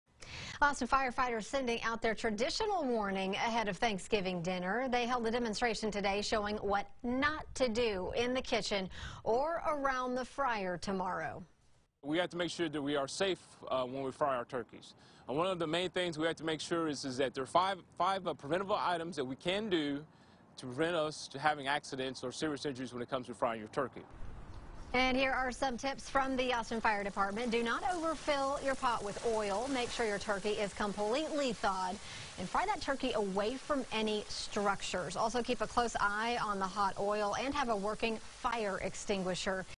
AFD demonstrates how 'not' to fry a turkey on Thanksgiving
Here are some tips from the Austin Fire Department.